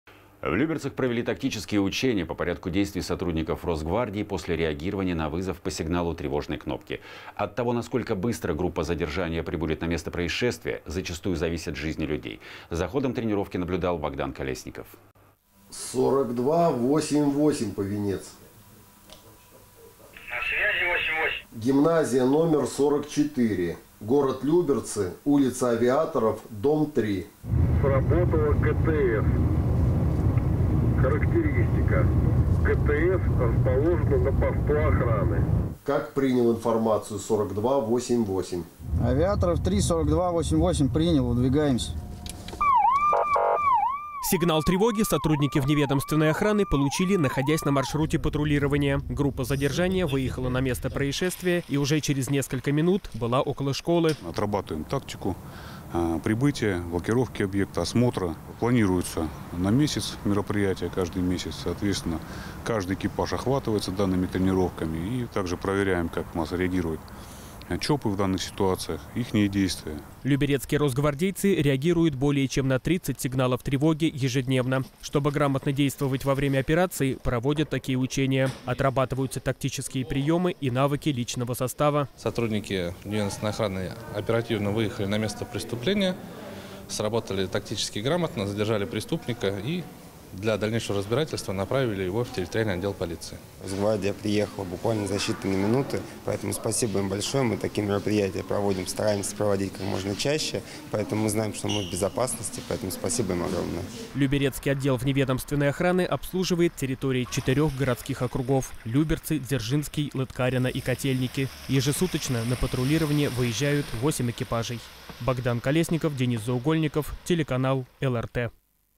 Тактические учения сотрудников Росгвардии
В Люберцах провели тактические учения по порядку действий сотрудников Росгвардии после реагирования на вызов по сигналу тревожной кнопки. От того, насколько быстро группа задержания прибудет на место происшествия, зачастую зависят жизни людей. За ходом тренировки наблюдала наша съёмочная группа.

Сигнал тревоги сотрудники вневедомственной охраны получили, находясь на маршруте патрулирования. Группа задержания выехала на место происшествия и уже через несколько минут была около школы.

Владимир Гайструк, начальник Люберецкого отдела вневедомственной охраны Росгвардии по Московской области: «Отрабатываем тактику, прибытие, блокировку объекта, осмотра. Планируются на месяц мероприятия. Соответственно, каждый экипаж охватывается данными тренировками. Также проверяем, как у нас реагируют ЧОПы в данных ситуациях, их действия».

Люберецкие росгвардейцы реагируют более чем на 30 сигналов тревоги ежедневно. Чтобы грамотно действовать во время операции, проводят такие учения. Отрабатываются тактические приёмы и навыки личного состава.

Евгений Паперный, заместитель начальника Люберецкого отдела вневедомственной охраны Росгвардии по Московской области: «Сотрудники вневедомственной охраны оперативно выехали на место преступления. Сработали тактически грамотно, задержали преступника и для дальнейшего разбирательства направили его в территориальный отдел полиции».

Дмитрий Викулов, директор гимназии № 44 г.о. Люберцы: «Росгвардия приехала буквально за считанные минуты. Спасибо им большое. Мы такие мероприятия стараемся проводить как можно чаще. Мы знаем, что мы в безопасности, поэтому спасибо им огромное».

Люберецкий отдел вневедомственной охраны обслуживает территории четырёх городских округов: Люберцы, Дзержинский, Лыткарино и Котельники. Ежесуточно на патрулирование выезжают восемь экипажей.